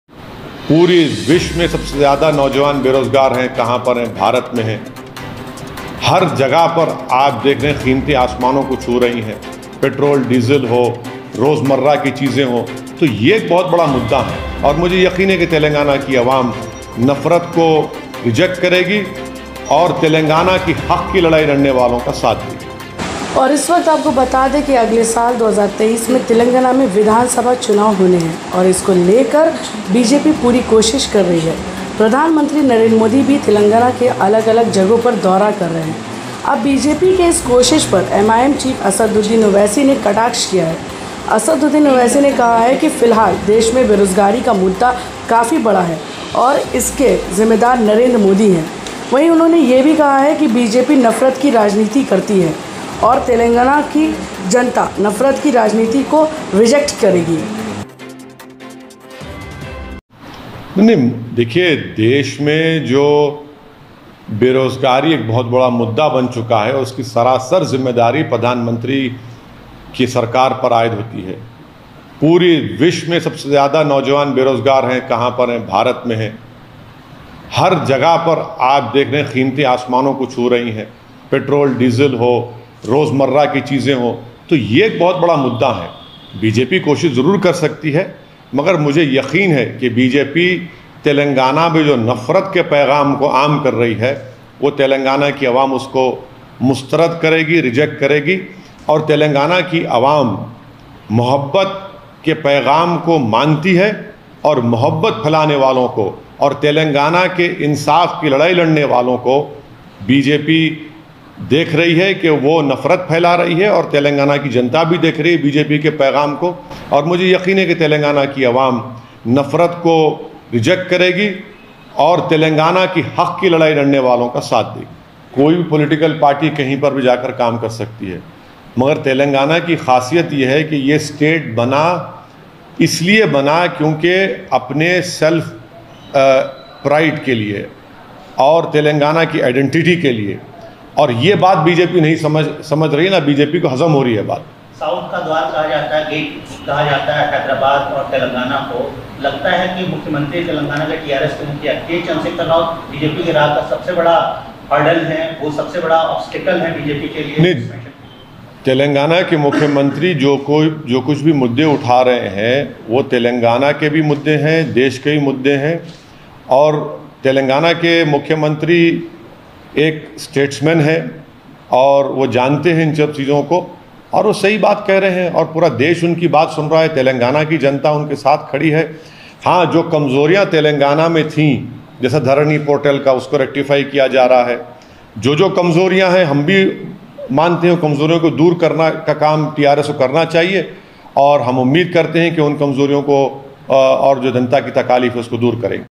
Telangana की जनता नफरत फ़ैलाने वालों को reject करेगी: Asaduddin Owaisi, AIMIM
Telangana की जनता नफरत फ़ैलाने वालों को reject करेगी: Asaduddin Owaisi, AIMIM | Live India News